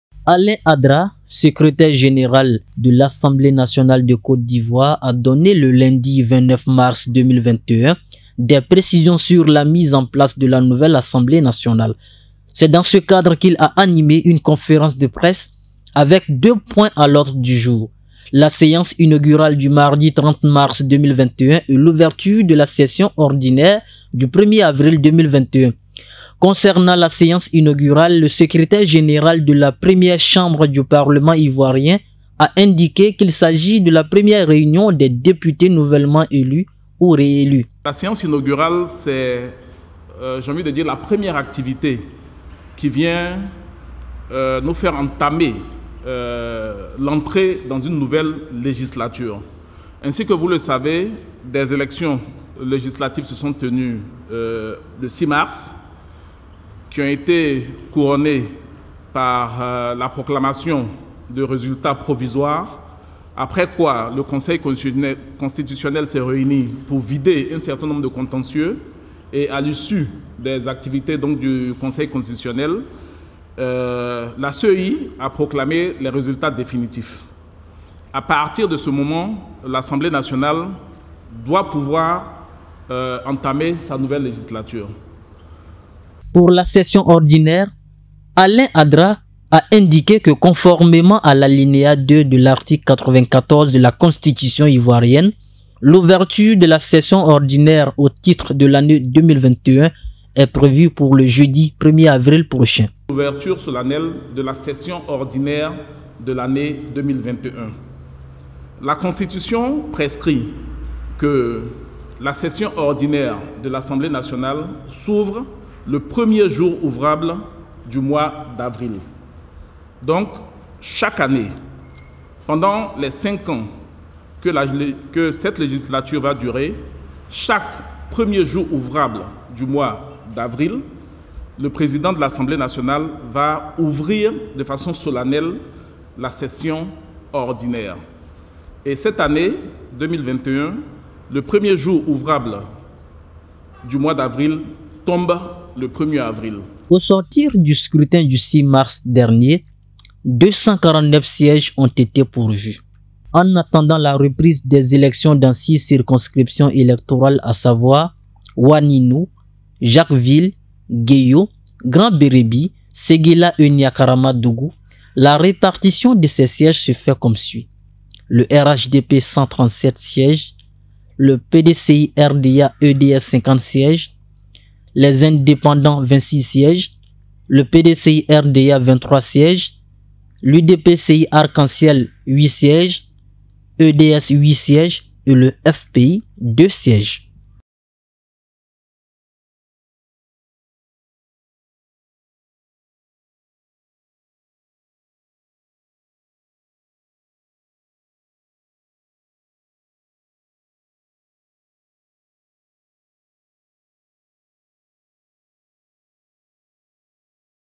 LES PRECISIONS DE ALAIN ADDRA SUR LA MISE EN PLACE DE LA NOUVELLE ASSEMBLEE NATIONALE
Le secrétaire général de l'assemblée nationale, Alain Addra a animé une conférence de presse ce lundi 29 mars 2021. L'objectif était de donner des précisions sur la nouvelle chambre haute du parlement ivoirien.

LINTELLIGENT.TV